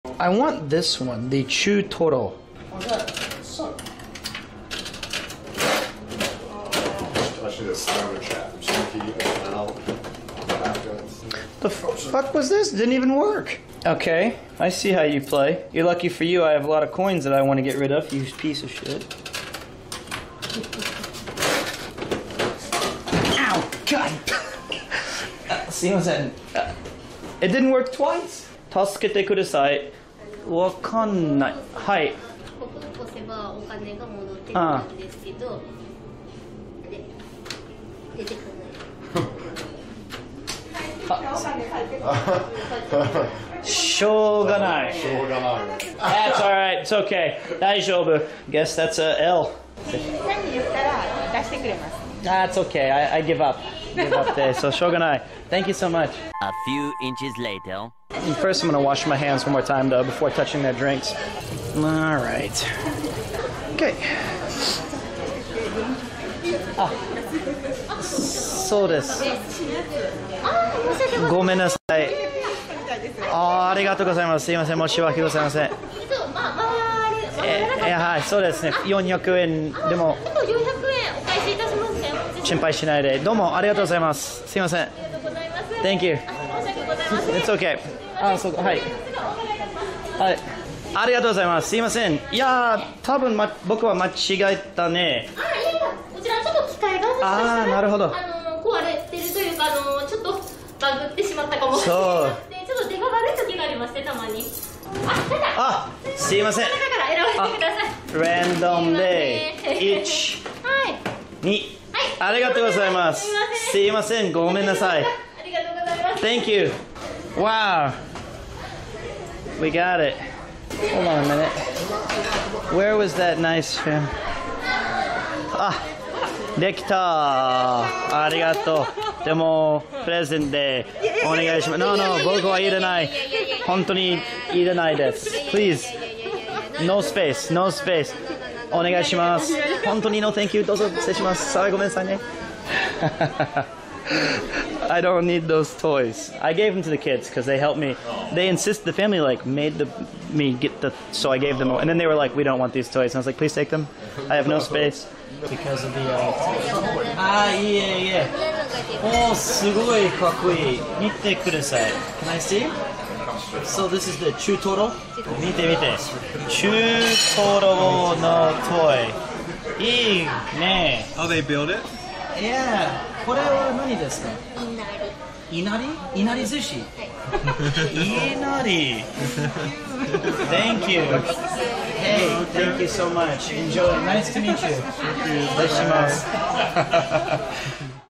Japanese family helps Pete from getting scammed, in return Pete did this
Follow Premier Two (Peter)!

Follow CdawgVA (Connor)!

Follow Abroad in Japan (Chris)!